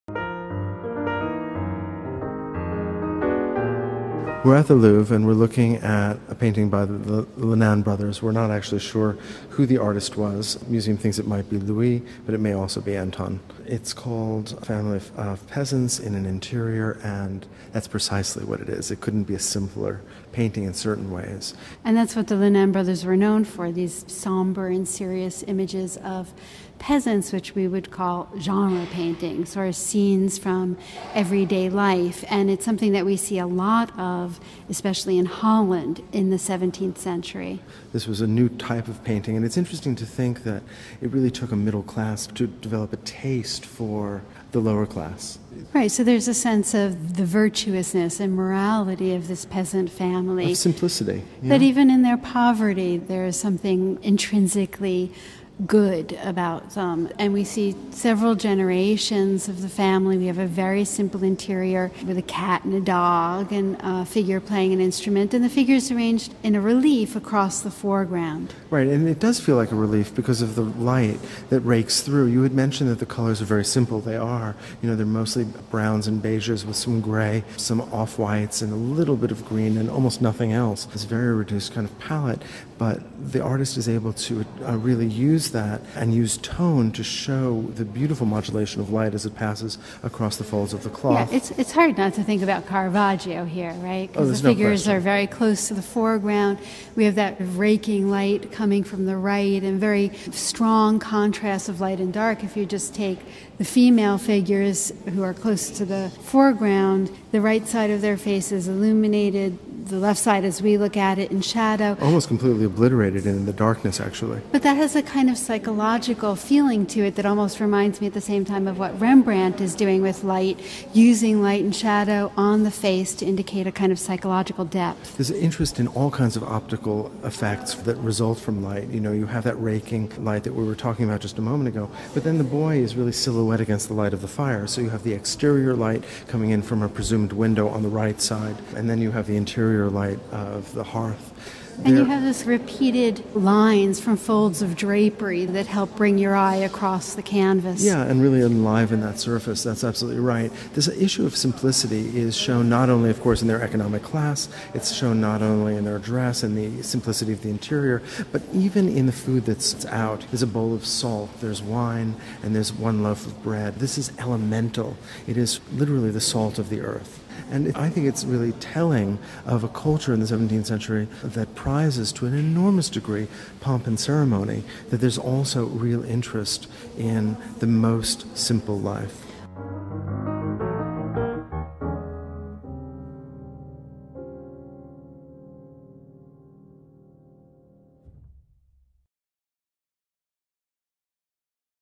Antoine or Louis Le Nain, Peasant Family in an Interior
Antoine or Louis Le Nain, Peasant Family in an Interior, 2nd quarter of the 17th century, oil on canvas, 1.13 x 1.59 m (Musée du Louvre, Paris). Speakers: Drs. Beth Harris and Steven Zucker. Created by Beth Harris and Steven Zucker.